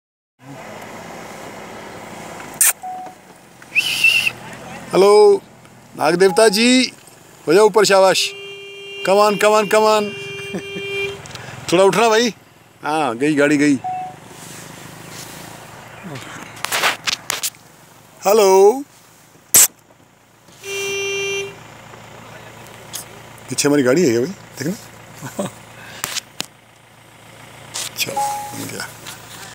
Cobra Snake
I was going to Shah jahan pur, passing the village Powayan,
the Road was blocked due to presence of a deadly snake's presence on the road. I was in a swift car.
My friend was driving the car. He also stopped the car.
Then I took a bold decision, I took the steering wheel of the cat & went very near to that Cobra snake.
I opened the window glass of my Car.
Then what happened, see yourself.